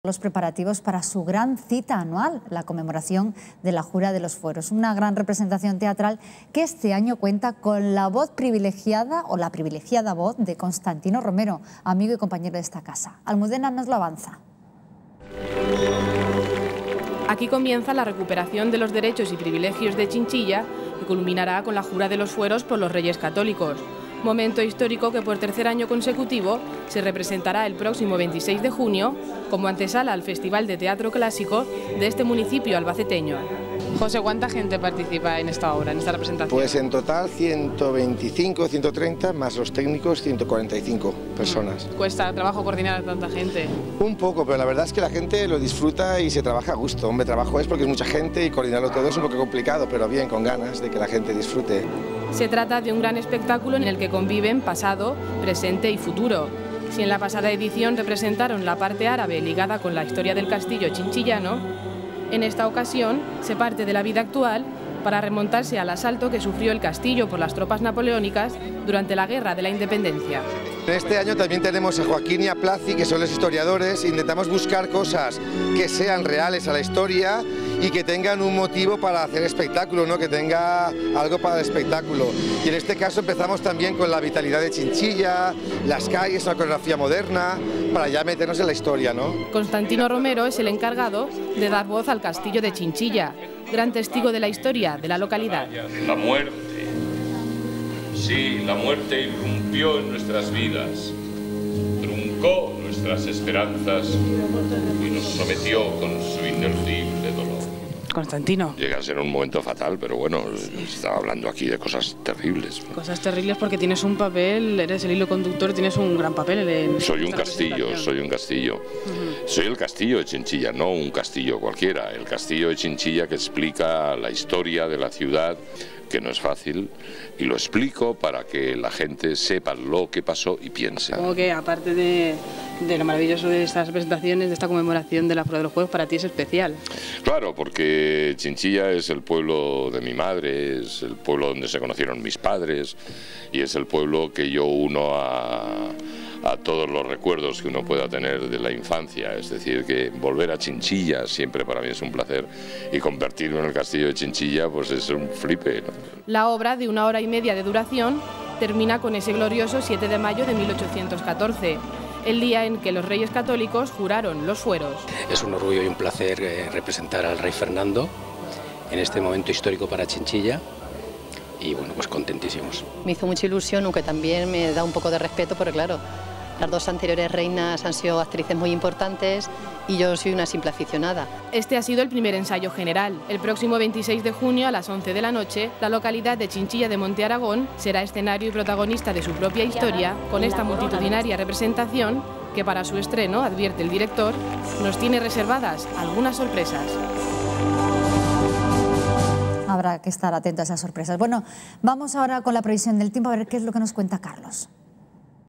Reportaje ensayo Jura Fueros de Chinchilla (Albacete)
Video sobre los ensayos anteriores a la Jura de los Fueros en la ciudad de Chinchilla (Albacete) por los Reyes Católicos. Acto que se celebrará el día 26 de Junio de 2010 a las 23:00h. Es una obra de la Escuela Municipal de Teatro de Chinchilla bajo la dirección de José Tomás Cháfer y con la presencia de Constantino Romero. Más sobre la escuela en Facebook